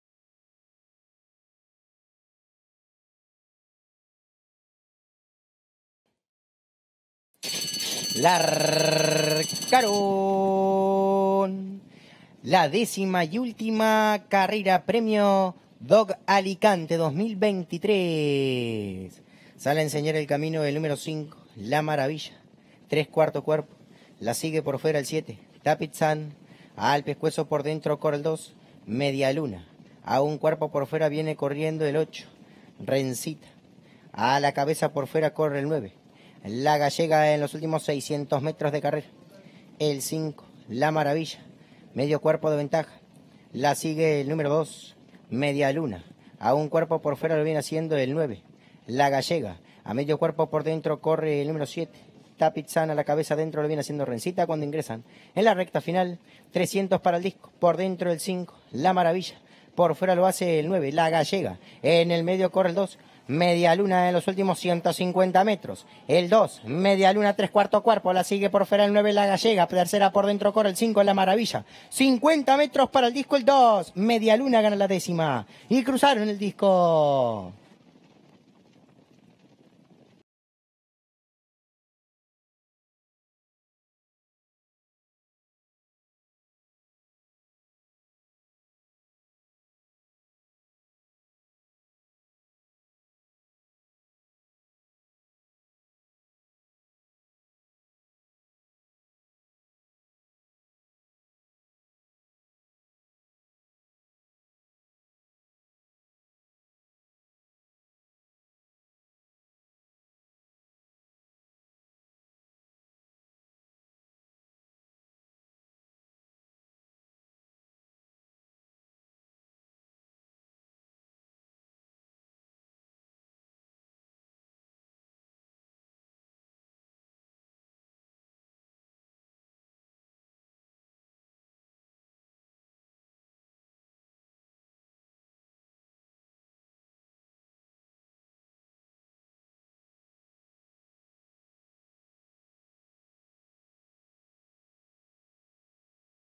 02/08/2024 – Hipodromo Las Piedras – Carr 10 – DOG ALICANTE (2023)
Carrera 10 realizada el dia 02/08/2024 por el premio DOG ALICANTE (2023) en el Hipodromo Las Piedras